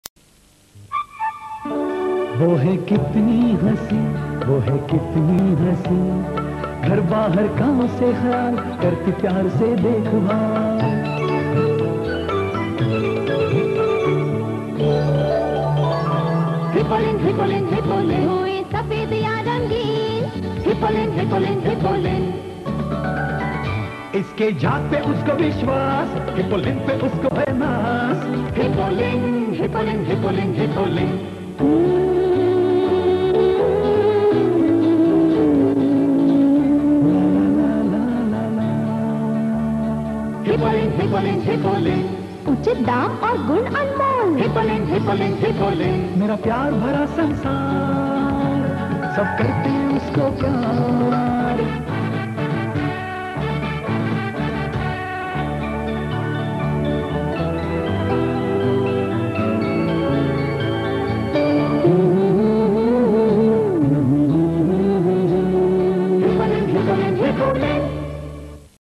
Hipolin | Keith Stevenson | Humayunn Niaz Ahmed Peerzaada
kahan kahan se guzar gaya. Memories of a beautiful journey as Model & Actor in front of camera from 1986 to 2007) & hopefully will start once again soon.